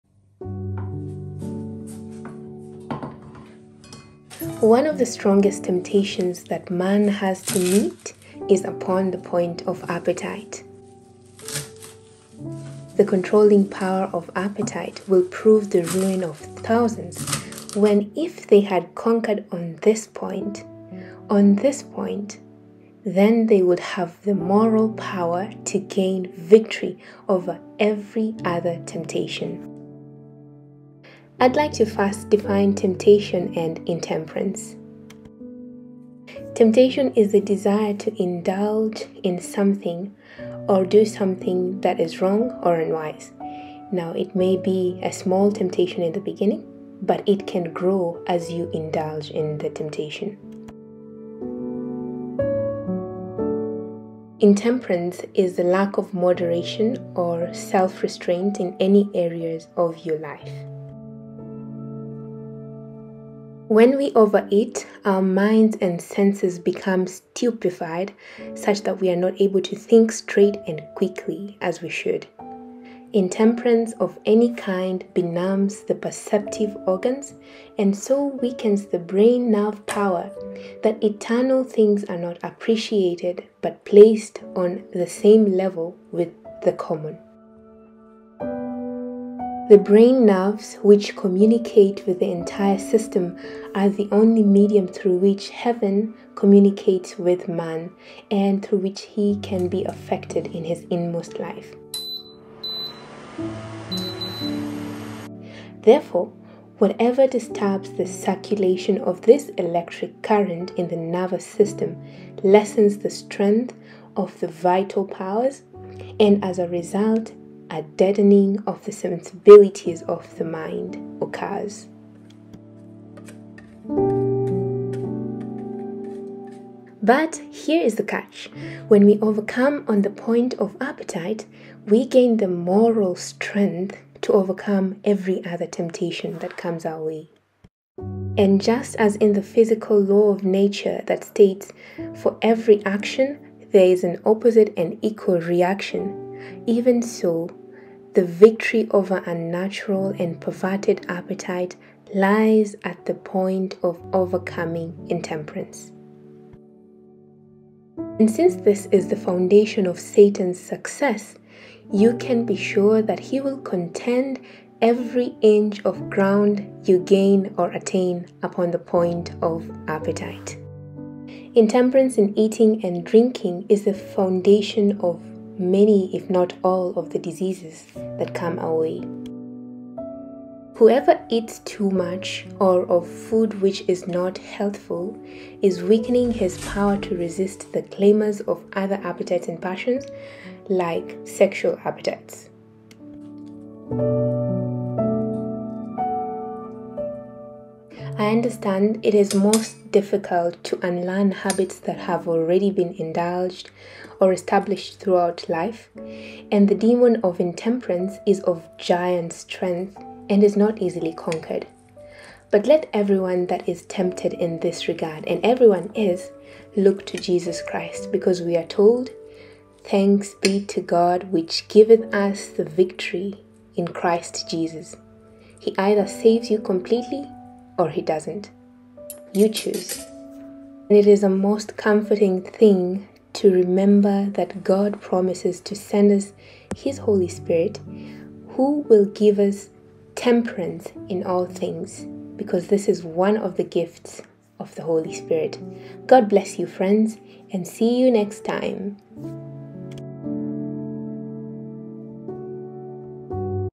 A vegan Omelette + The basis of overcoming temptation
"The controlling power of appetite will prove the ruin of thousands, when, if they had conquered on this point, they would have had moral power to gain the victory over every other temptation of Satan."

Omelette Recipe:

Ingredients:

For the batter:
🌱Gram flour
🌱Turmeric
🌱Black salt (Kala namak)
🌱Water

For the aromatics:
🌱Coriander
🌱Onion
🌱Garlic
🌱Ginger

For Garnish:
🌱Tomatoes
🌱Mushroom (Optional)
🌱Some more coriander

1. Prepare the batter by mixing the batter ingredients.
2. Add in all the aromatics. Whisk well then cook on the stove.
3. Flip to fold when the top of the omelette is dry. Cook for a few more minutes.
4. Serve with some rice, bread of a meal of choice!

Happy overcoming Friends!
Barb💚